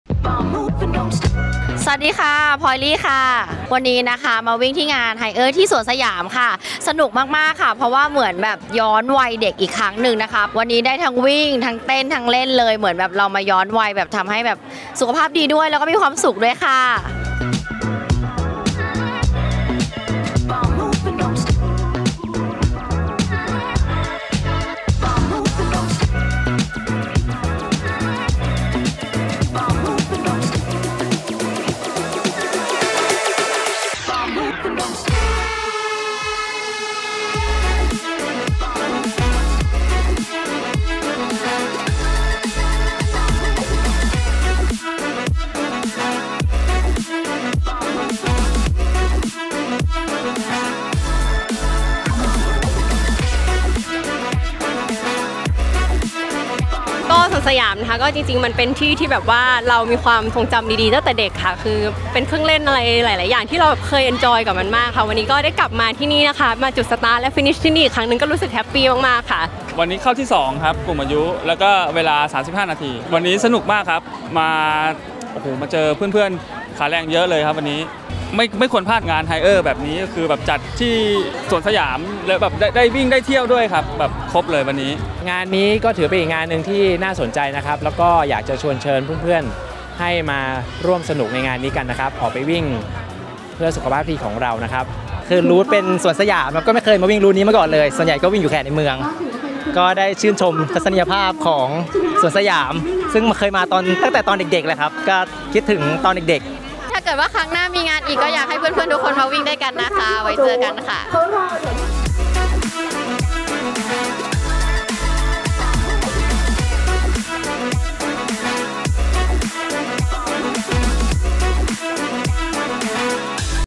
บรรยากาศงาน Haier Run 2023 "Inspire Future Dreams” พร้อมบทสัมภาษณ์เหล่านางฟ้านักวิ่ง
🏃งาน Haier Run 2023 "Inspire Future Dreams” ที่ สยามอะเมซิ่งพาร์ค | Siam Amazing Park
👉งานนี้นอกจากวิ่งแล้วได้บุญ เพื่อนๆก็บอกเป็นเสียเดียวกันว่าฟินกับการวิ่งผ่านเครื่องเล่นสุดอลังการในสวนสยาม แถมเหรียญก็สวยงามอีกต่างหาก ความฟินยังไม่จบเท่านี้ เพราะวิ่งเสร็จก็มีมินิคอนเสิร์ตจากแดน-บีมให้สนุกอีกด้วย คิดดูว่าฟินแค่ไหน
👉ใครพลาดงานปีนี้...ปีหน้าต้องมาให้ได้นะครับ

ฝากติดตาม Fanpage RUNไปเรื่อยๆ
เพื่อไม่ให้พลาดทุกข่าวสารวงการวิ่ง ทุกเทคนิคการวิ่งด้วยครับ